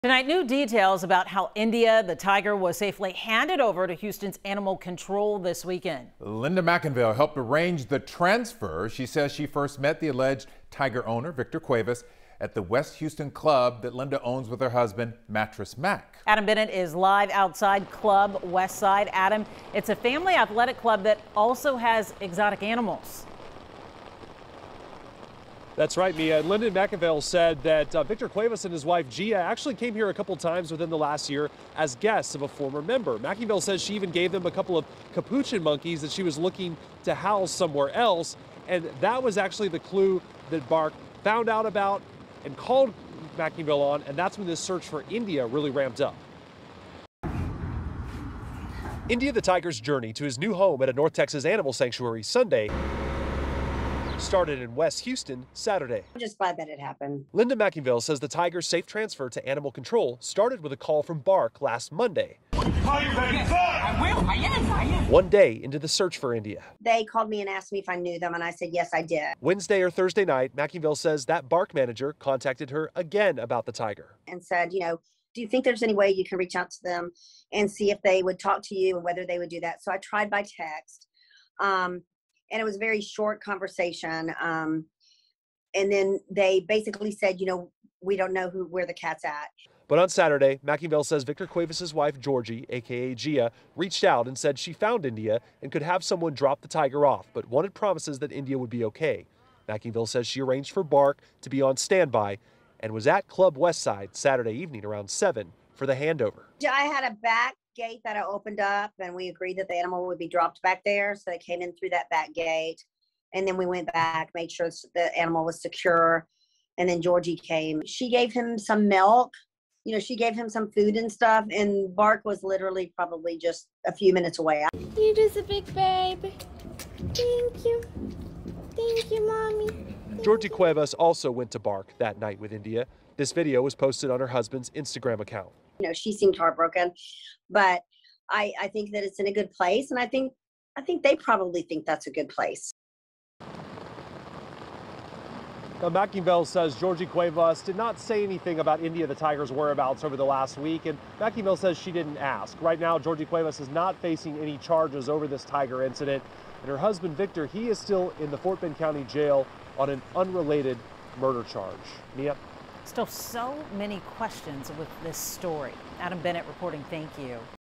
Linda McIngvale reveals more details about India the tiger surrender
New details are emerging about how India the tiger was safely handed over to Houston’s animal control over the weekend.